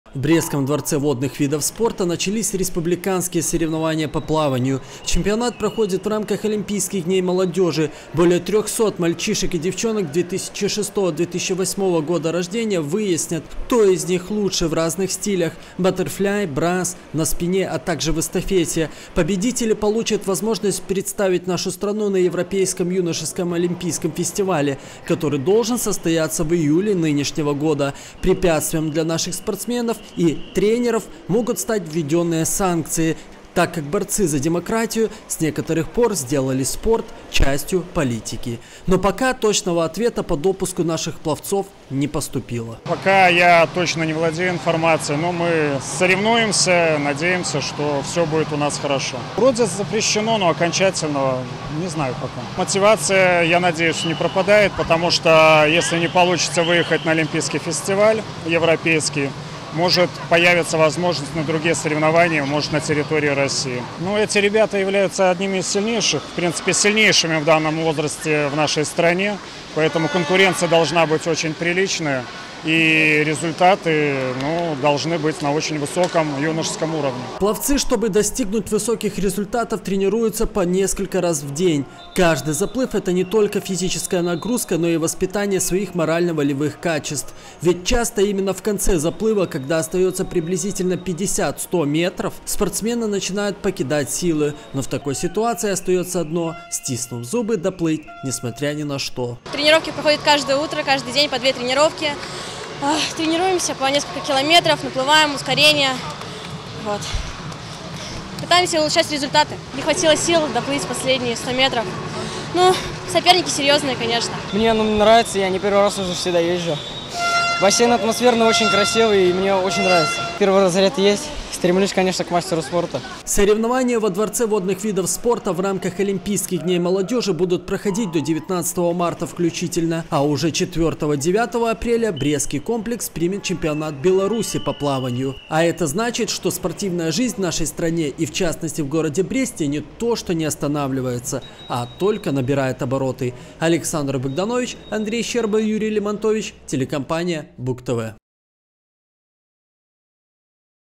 2022-03-16 г. Брест. Республиканские соревнования по плаванию. Новости на Буг-ТВ.
В Бресте проходят Республиканские соревнования по плаванию в рамках олимпийских дней молодёжи. Со всей страны в город над Бугом съехались порядка трехсот спортсменов. На протяжении 4-х дней ребята между собой  будут определять сильнейших в плавании вольным стилем, баттерфляем, брассом, на спине, комплексном плавании, на дистанциях от 50 до тысячи пятисот  метров. Эти Республиканские соревнования являются отборочными для участия в Европейском юношеском олимпийском фестивале, который должен состояться в июле нынешнего года.